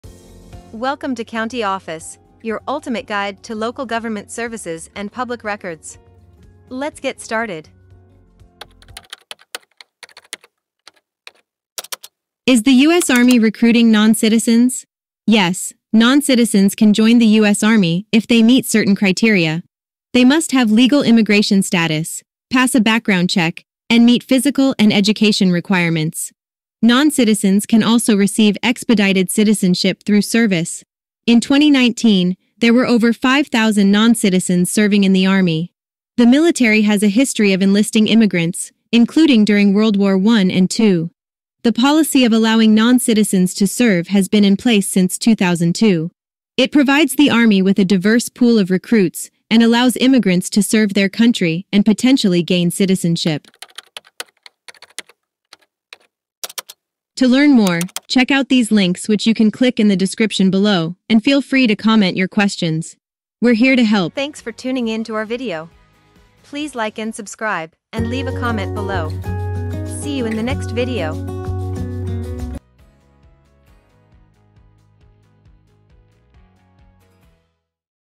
Is The US Army Recruiting Non-Citizens? - CountyOffice.org
Is The US Army Recruiting Non-Citizens? Did you know that non-citizens can join the US Army? In this eye-opening video, we'll explore the criteria for non-citizens to join the US Army, including legal immigration status, background checks, and physical and education requirements. We'll also discuss how non-citizens can receive expedited citizenship through service and the benefits of enlisting immigrants.

With over 5,000 non-citizens serving in the Army in 2019, it's clear that the military has a history of enlisting immigrants, including during World War I and II. The policy of allowing non-citizens to serve has been in place since 2002, providing the Army with a diverse pool of recruits and allowing immigrants to serve their country and potentially gain citizenship.

If you're interested in joining the US Army as a non-citizen, or simply curious about the process, this video is a must-watch. We'll also provide helpful resources, including CountyOffice for military base locations and the US Army website for more information on who they are and what they do.